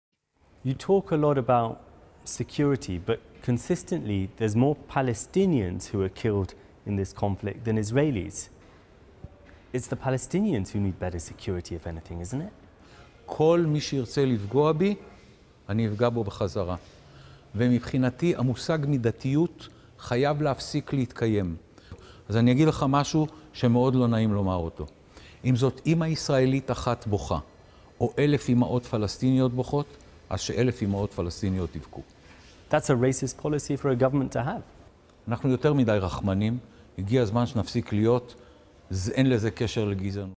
"1,000 Palestinian mothers will cry": Far-right Israeli politician's shocking statement
Far-right Israeli member of Knesset Zvika Fogel's disturbing response to Channel 4 Foreign Correspondent, Secunder Kermani, on how the new Israeli government intends to de-escalate the tension between Israelis and Palestinians in Israel and the occupied West Bank moving forward.